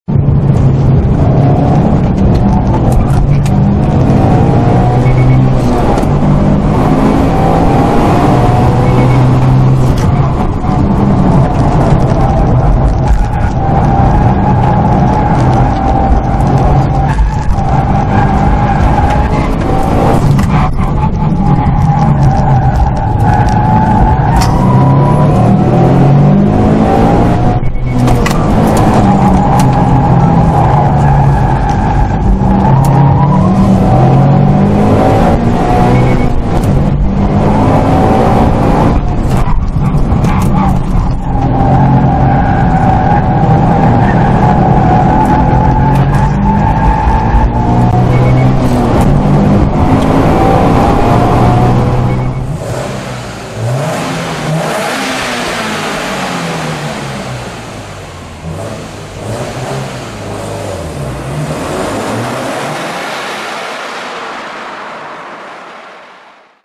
tc1000 43,050 WRX STI VAB PROVA PCC 2018-10-3
今回はPROVAの走行会PCC
にて走行した時のベストタイムです。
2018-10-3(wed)
天候-晴れ
気温-26度
コンディション-ドライ
タイヤ-SPORT MAXX 245/40/18 純正
サス-HKS SP
ウイング-varis gtウイング